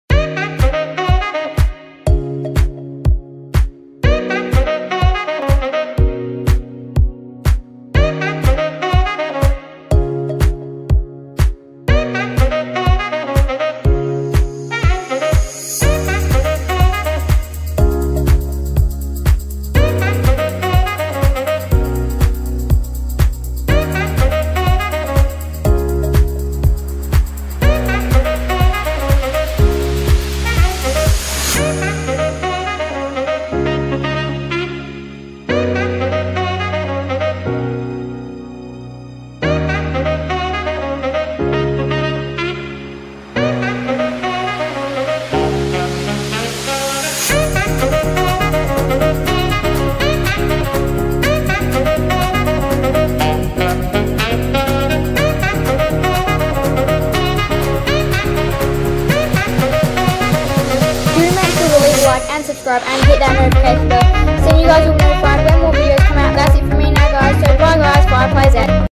AFL Round 1 Predictions!
Musician: LiQWYD
Hope you guys enjoyed my round 1 AFL tips! Whos excited for footy because I certainly am! So you never miss another video! Also there is something special coming in a few days time!